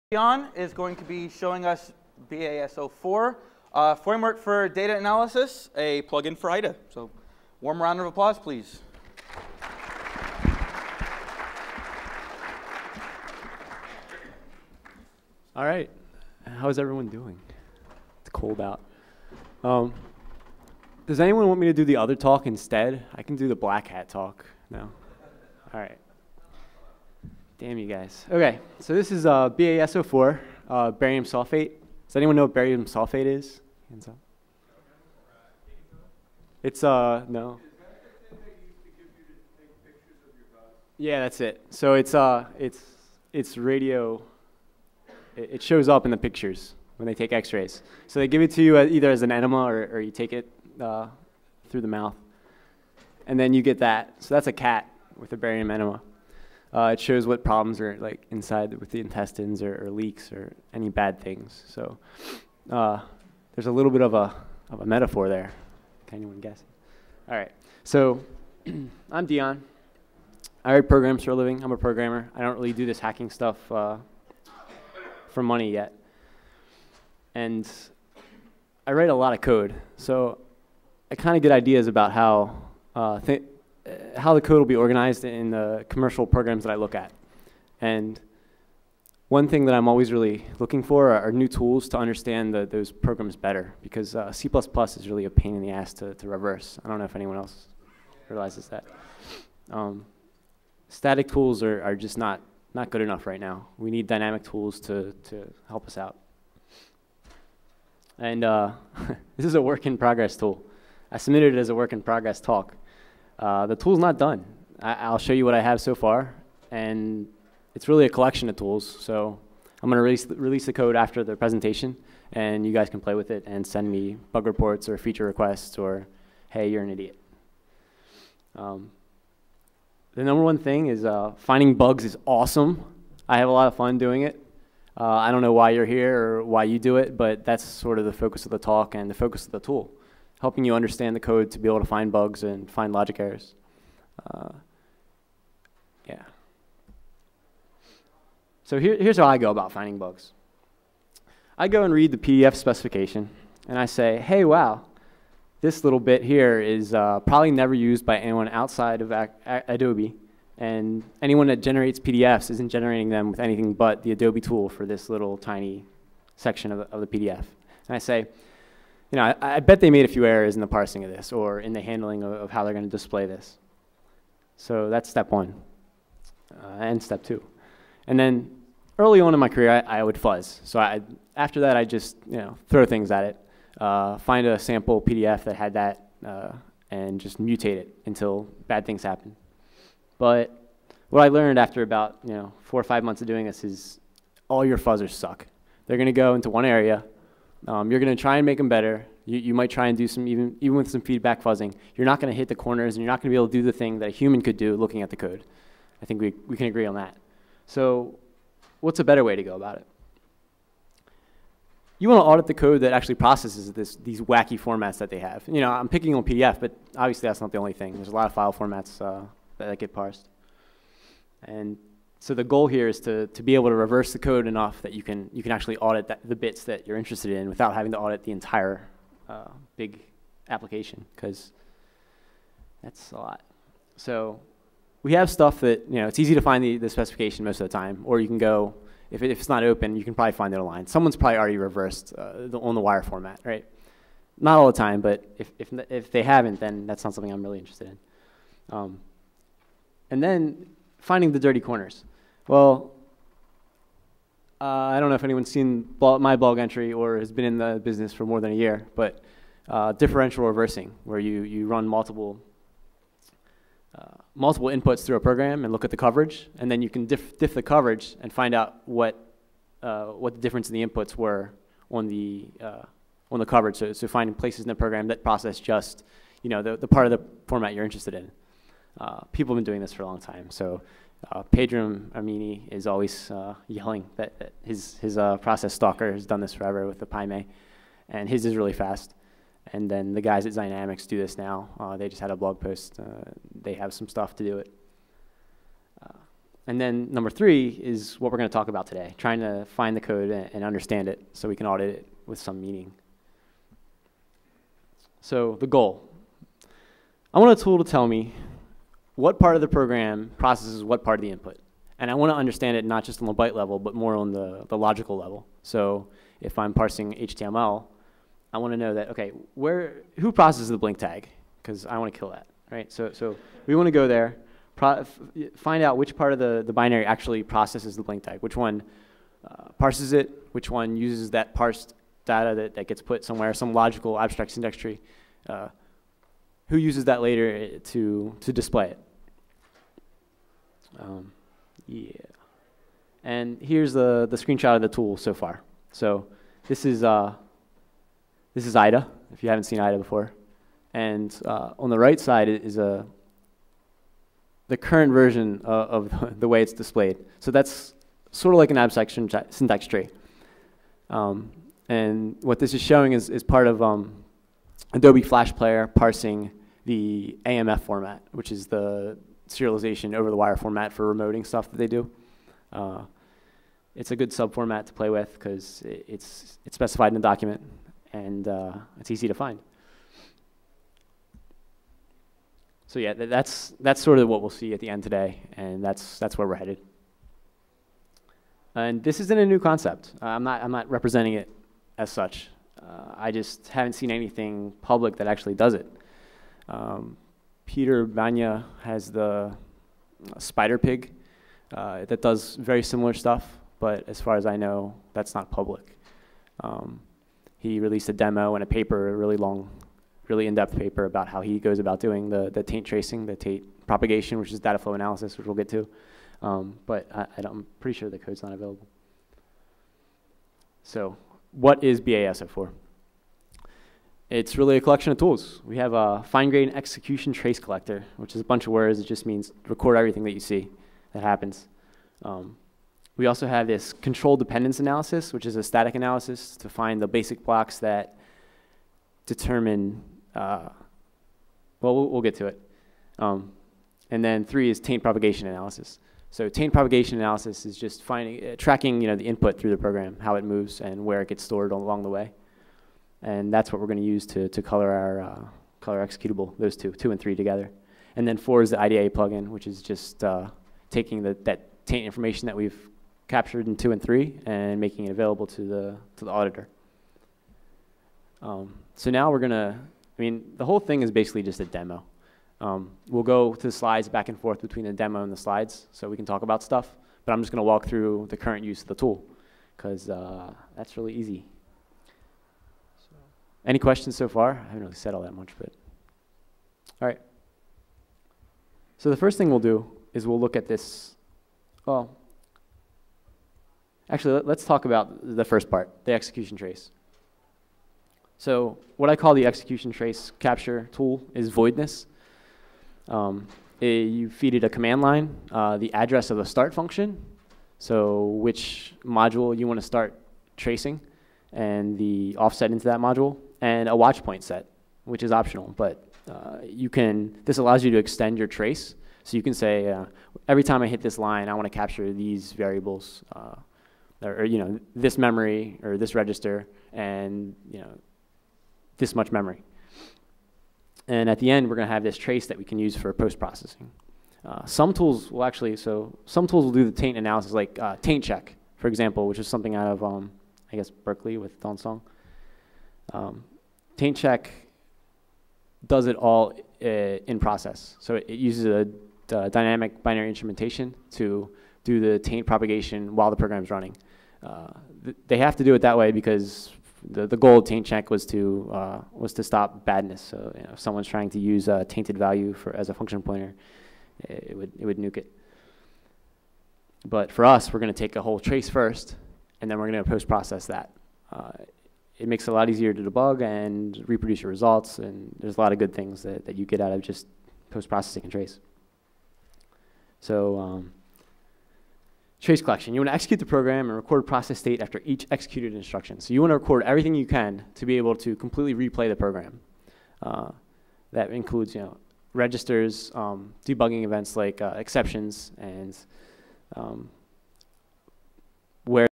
Shmoocon 2010: BaSO4: A Dynamic Dataflow Analysis Tool for Auditing and Reversing 1/5
Clip 1/5
Speaker: Dion Blazakis

The complexity of modern applications makes binary auditing a long slow march without a significant investment in tools and techniques. BaSO4 is a new IDA plug-in that highlights the instructions responsible for processing and propogating the information stored at a given input range. Using dynamic dataflow analysis based on a captured execution trace, BaSO4 can compute, for example, the instructions, memory locations, and registers used to process the string table in a Flash SWF file.

This information can be used to target manual audits and assist in reverse engineering. The analysis is computed for each byte of the tainted input and is linked to the abstract syntax of the input files. The IDA plug-in switches between various levels of abstract syntax and dynamically updates the highlighted code regions. The Tamarin VM (Adobe's open source AVM2 bytecode engine) is used as a case study to illustrate the strengths and weaknesses of BaSO4.